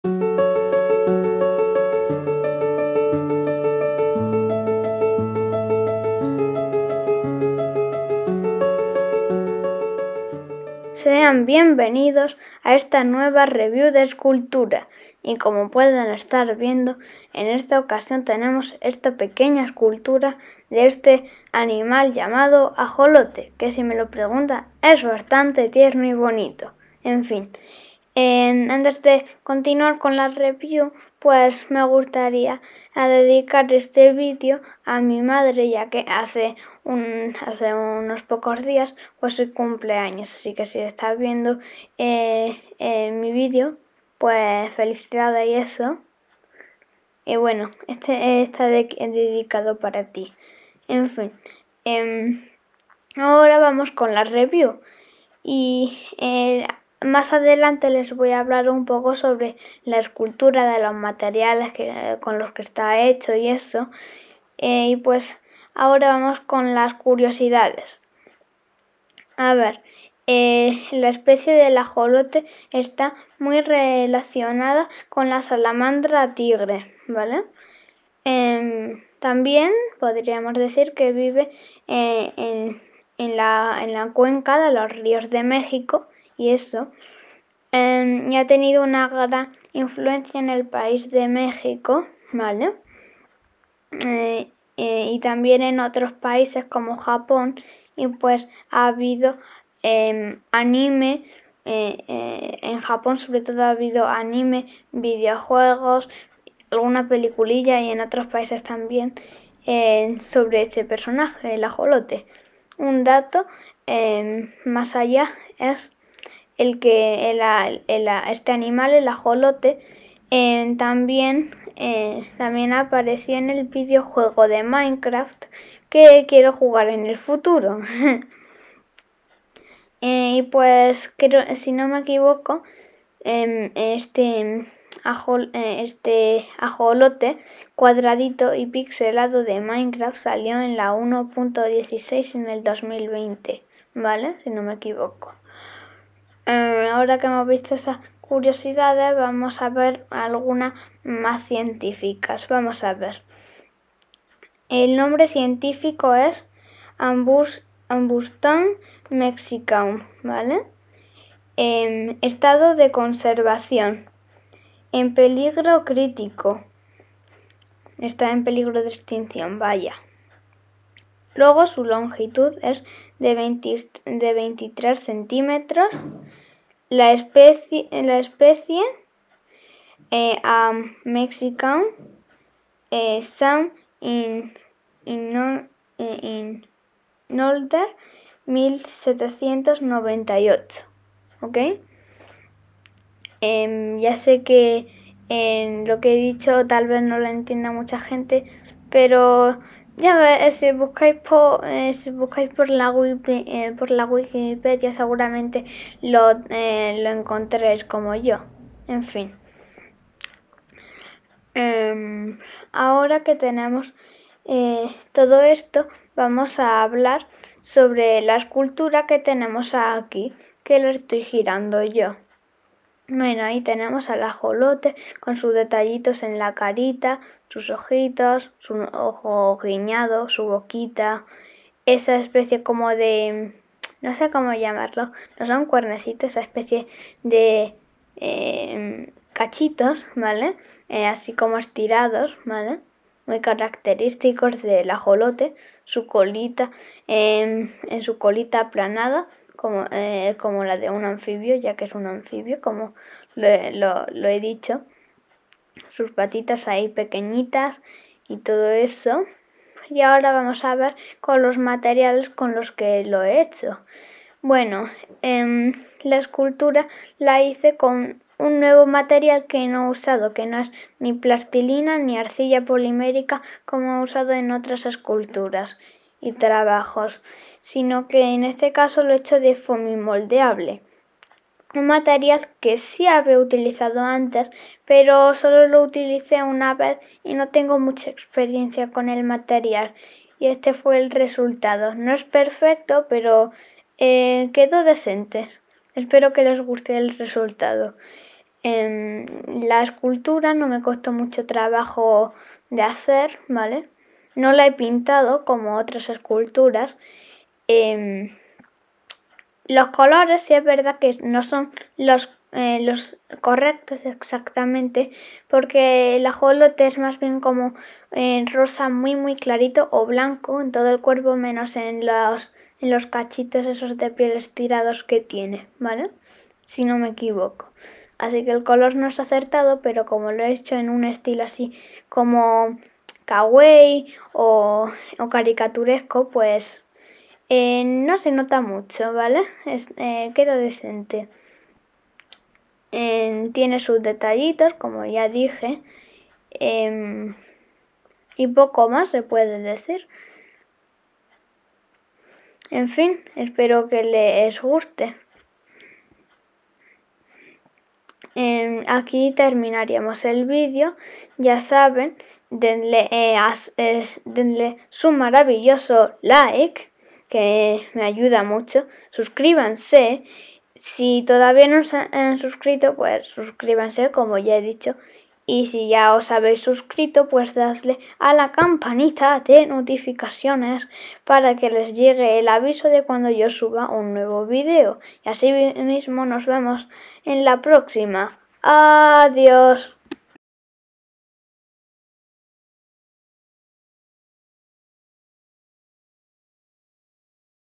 Review de escultura figura de Ajolote
Hoy vemos una escultura kawaii de un Ajolote, comento algo sobre esta especie animal tan curiosa, y cómo he realizado la figura.